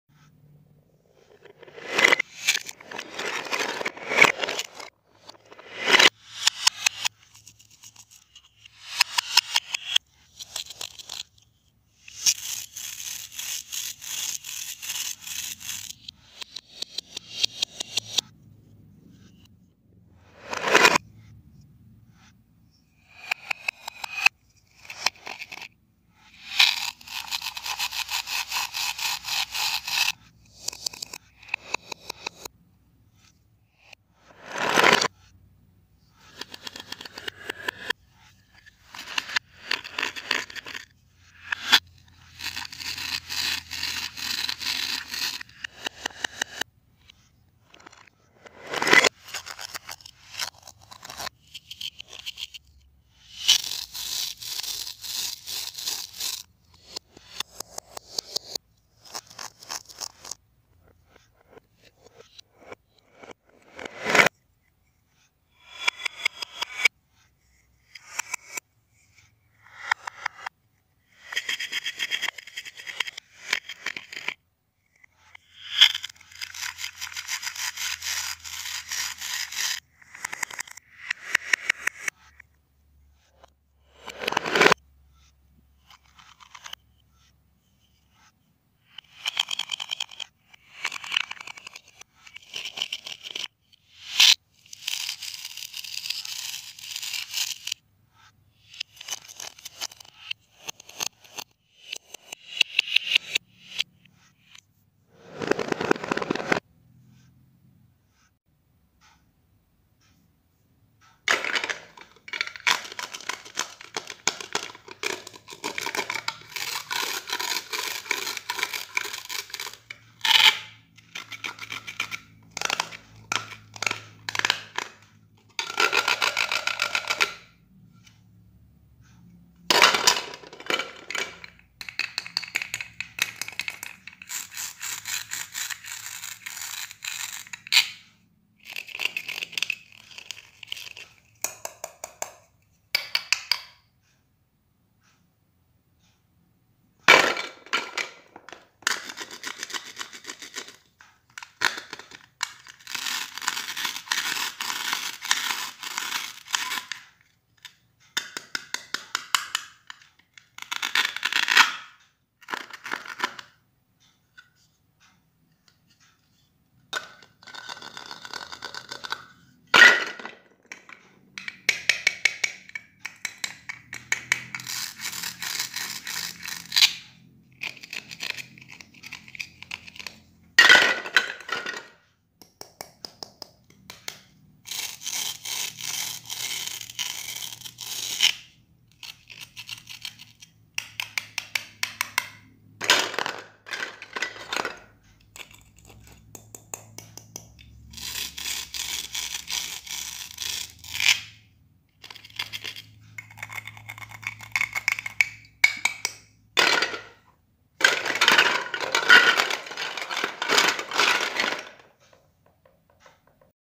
Satisfying Video | ASMR Cutting Fruits And Vegetables tomato carrot grape Apple broccoli corn kiwi
Welcome to the kids funny ASMR youtube channel.

This video is a satisfying Asmr video, by watching this video you can be calm and happy, satisfying fruit and vegetable videos can make you more relaxed.

From this satisfying video we can learn the names of fruit and vegetable names and learn the names of the colors of fruits and vegetables. I hope this satisfying video is useful for all of you.

Kids Funny ASMR